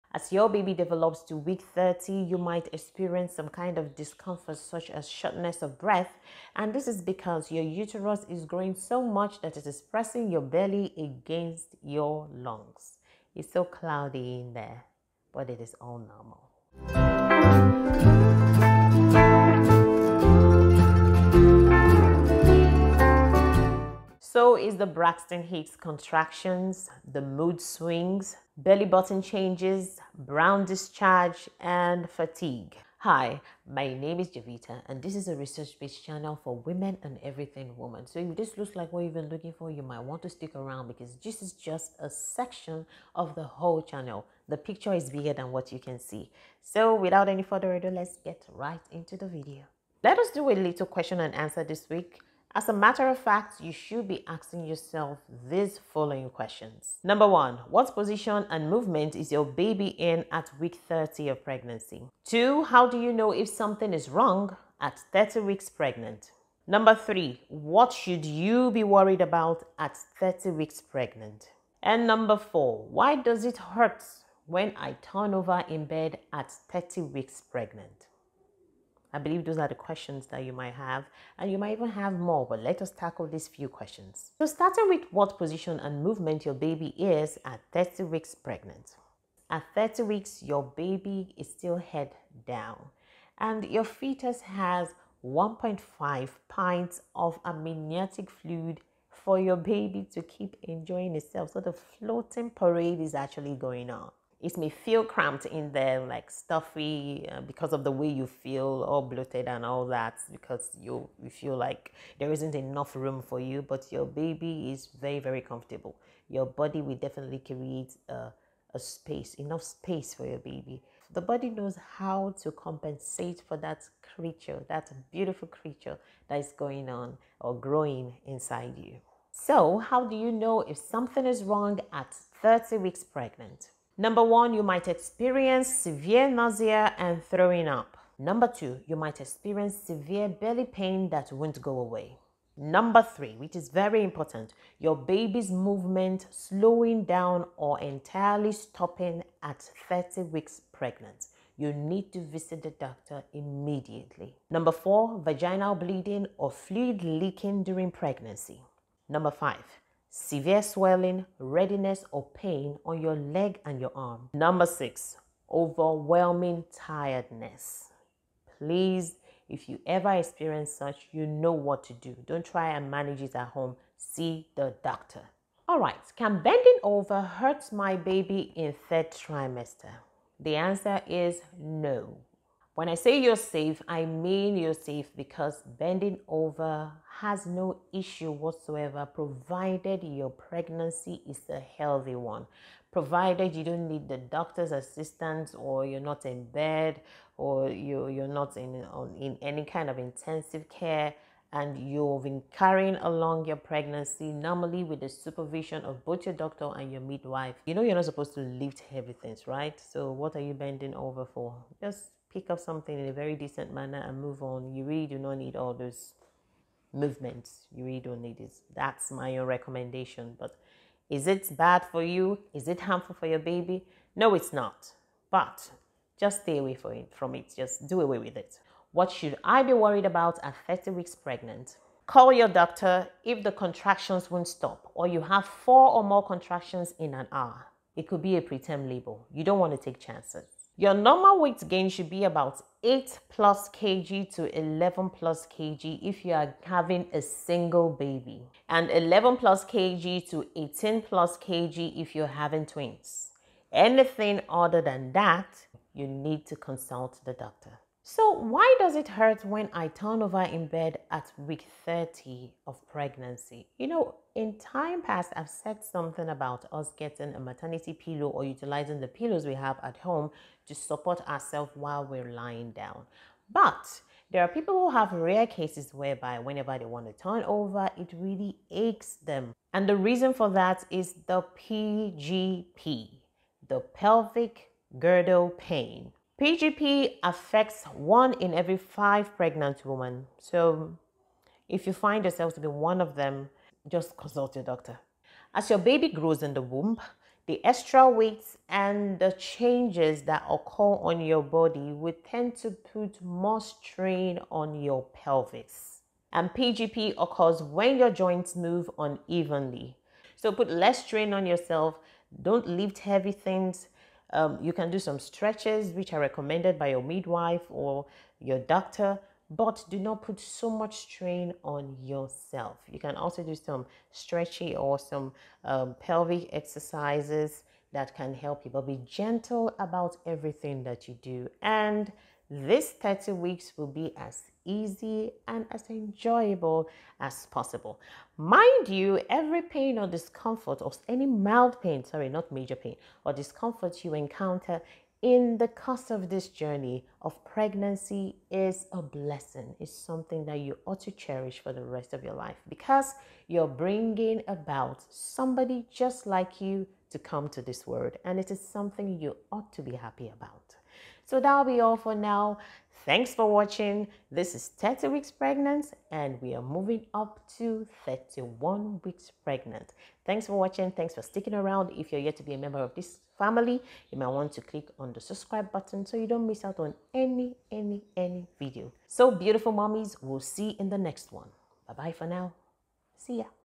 Crucial Pregnancy Questions to guide you at week 30
Pregnancy week 30 is a big step, and I know you feel the journey is ending! Beautiful things take beautiful steps, and it's worth the wait. Congratulations!
Today, we will answer some crucial questions to help this week be hassle-free for you, and I hope this helps.